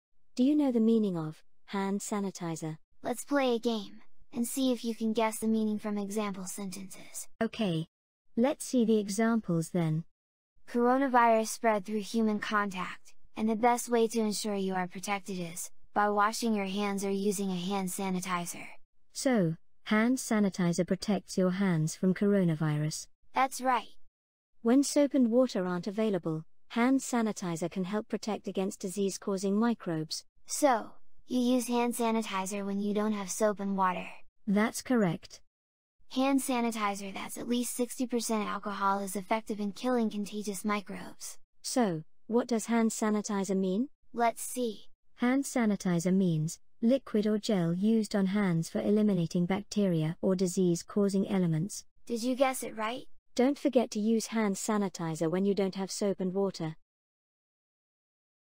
What does HAND SANITIZER mean? - Learn English with Sentences
Do you know what Hand Sanitizer means? Can you guess from example sentences? Let's play a game and see if you can guess it right!